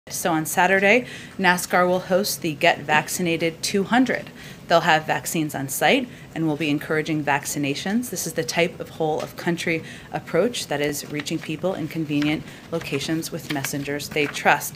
Jen Psaki announces that Nascar will have a 'Get Vaccinated 200' race on Saturday
Press Sec. Jen Psaki says NASCAR will host the 'Get Vaccinated 200' to encourage people to getvaccinated